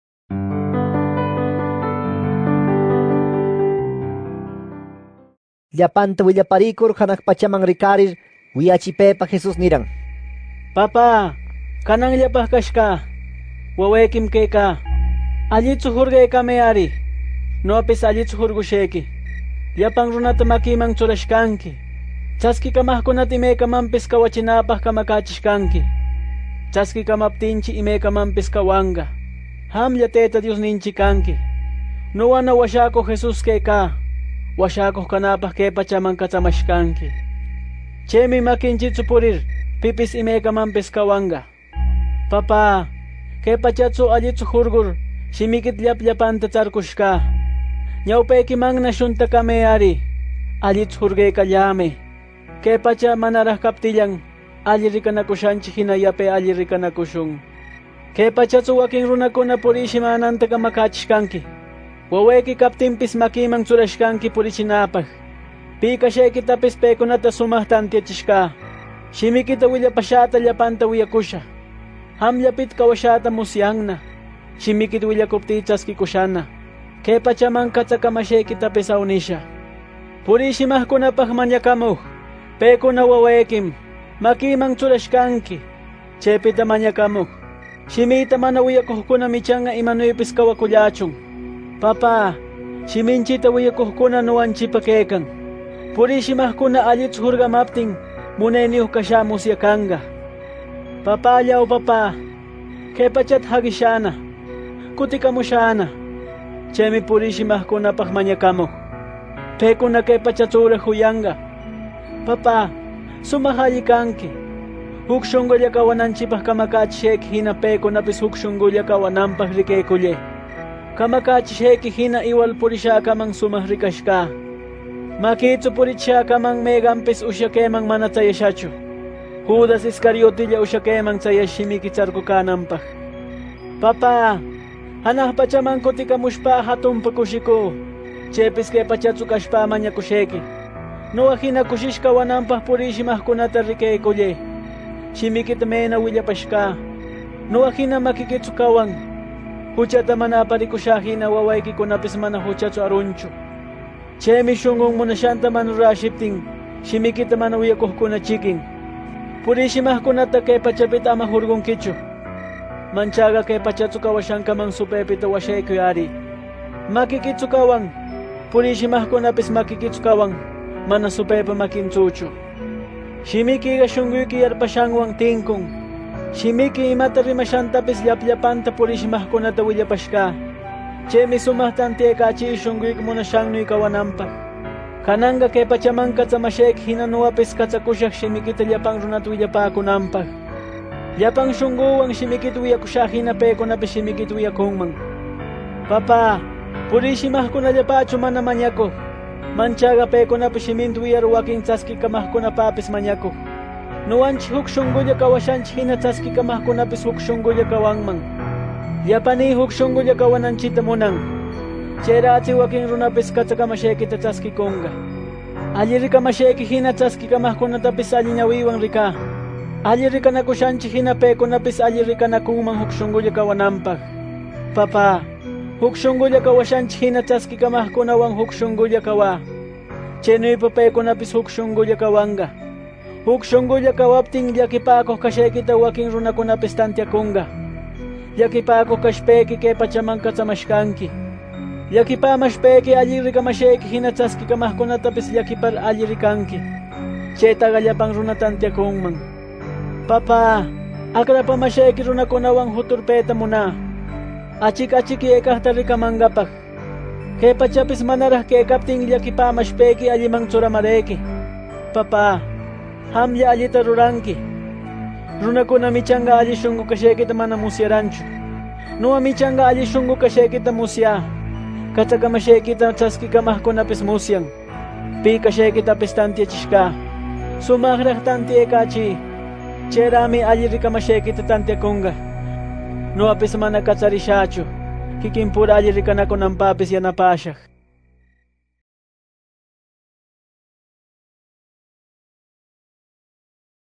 Quecha Panao Huánuco - John 17:1-26 - Jesus prays for his followers[qxh]
The video “Jesus prays for his followers” from the Gospel of John in the Quecha Panao Huánuco language of Peru [qxh] - John 17:1-26